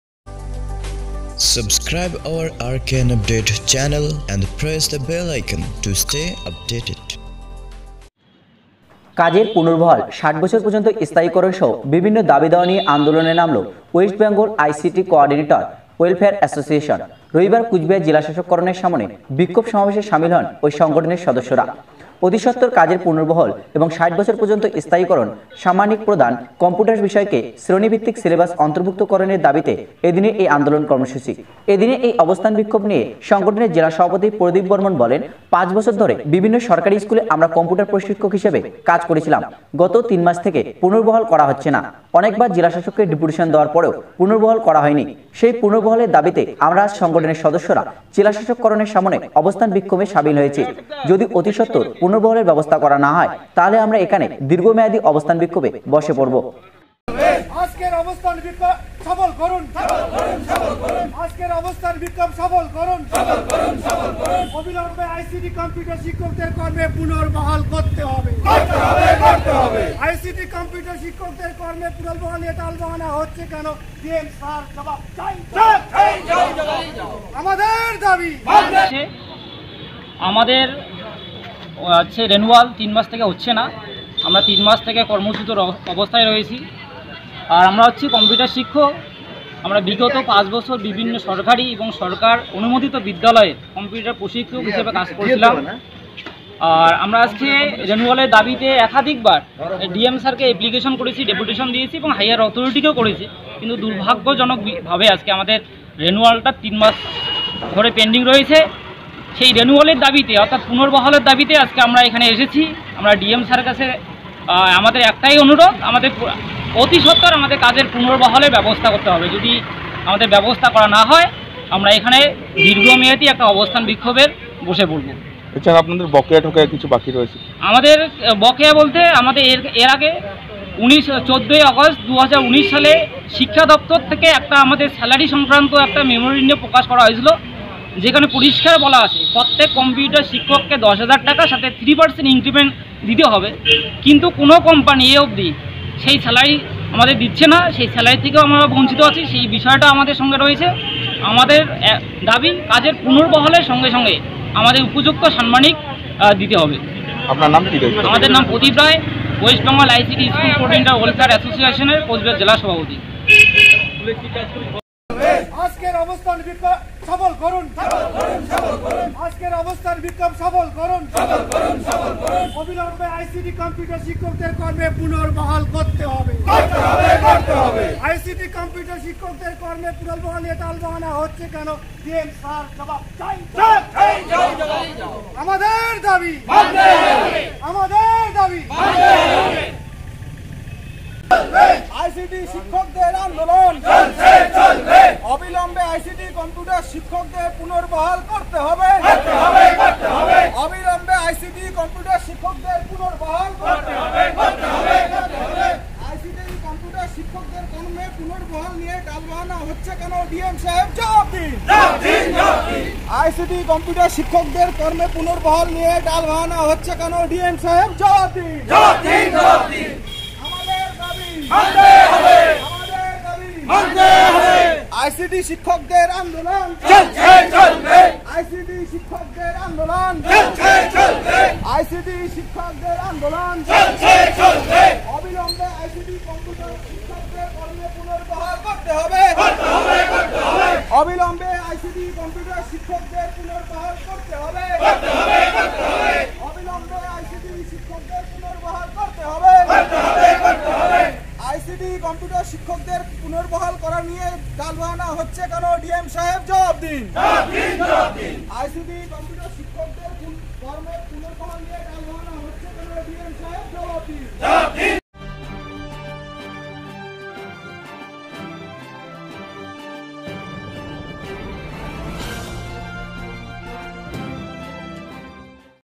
ICT শিক্ষকদের পুনর্বহালের দাবিতে কোচবিহার জেলা শাসকের সামনে অবস্থান-বিক্ষোভ
আইসিটি শিক্ষকদের পুনর্বহালের দাবিতে কুচবিহার জেলা শাসকের সামনে অবস্থান-বিক্ষোভ